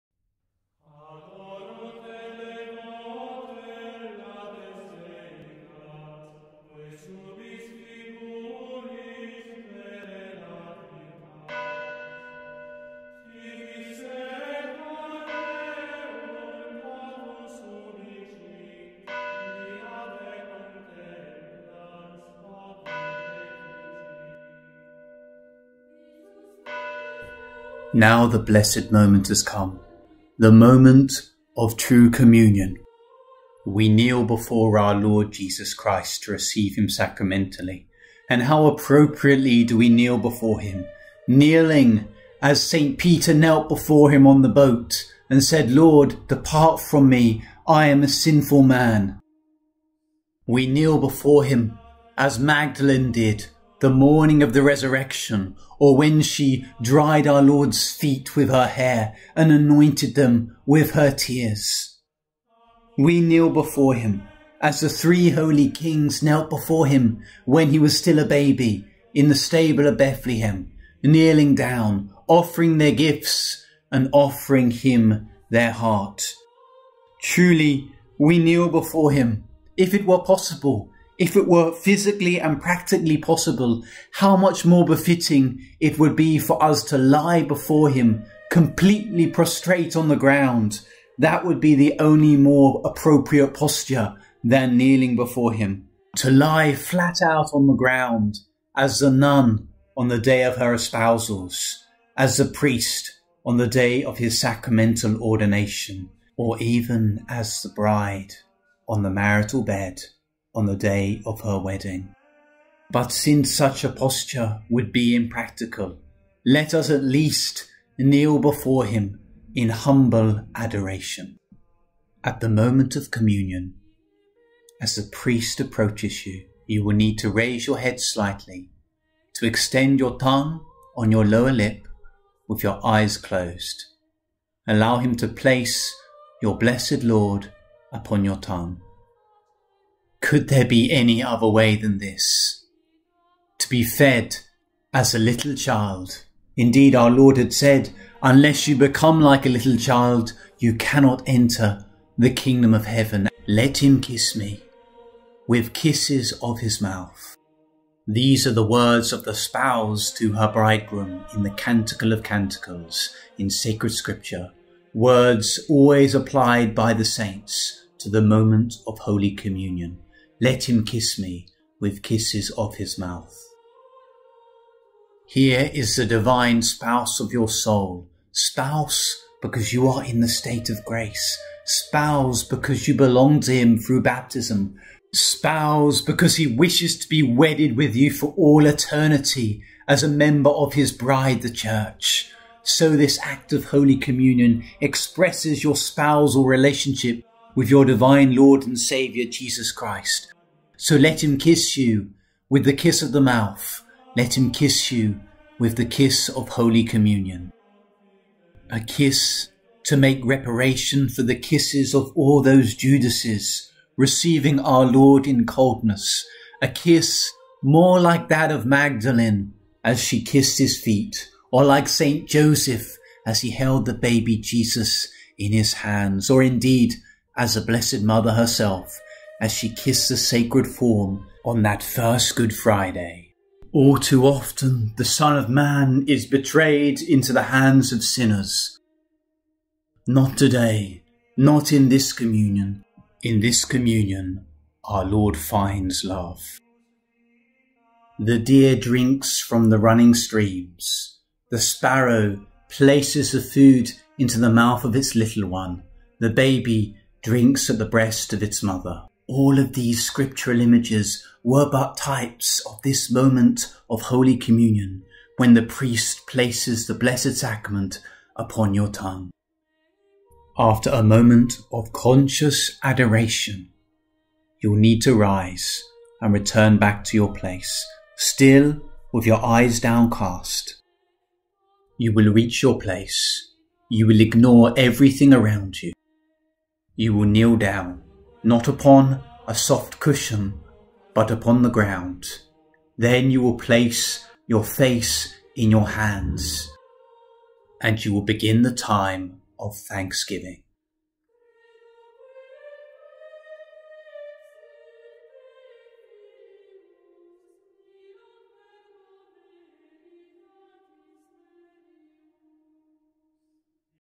Towards a DEVOUT reception of HOLY COMMUNION (Step by Step Guide) - The MOMENT of HOLY COMMUNION
In this series, I am presenting, step-by-step, a guide to receiving HOLY COMMUNION devoutly according to the teaching of the saints. So much has been forgotten and all too often people mistakenly reduce everything to a simply question of "Latin Mass or Novus Ordo" or "Hand or Tongue". These are important, but do not be distracted, there are many steps that need to be taken towards a devout reception of Holy Communion. Some you may not even be aware of because they are practices so forgotten and ignored, even by priests in traditional orders.

This episode focuses in on the moment of receiving Our Lord in Holy Communion.